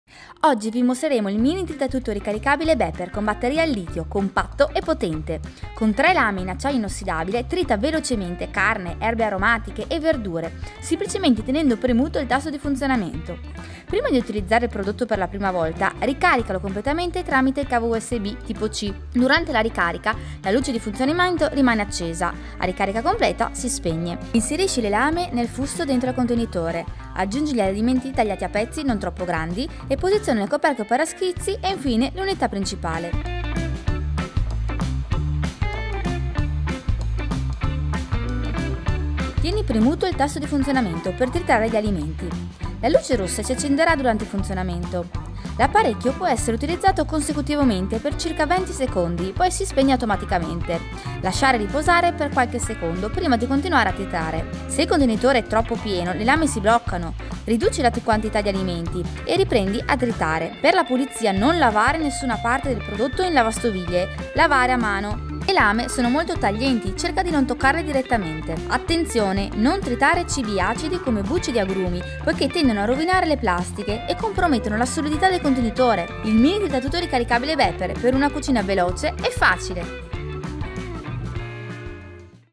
Mini tritatutto USB: l'elettrodomestico di cui non sapevi di aver bisogno!
Mini tritatutto ricaricabile con batteria al litio compatto e potente.
Il prodotto è dotato di tre lame in acciaio inossidabile che tritano velocemente carne, erbe aromatiche, verdure semplicemente tenendo premuto il tasto di funzionamento.
Contenitore da 250ml. Autonomia fino a 20 cicli con ricarica di 5 ore.
 Inclusa nella confezione un cavo USB per la ricarica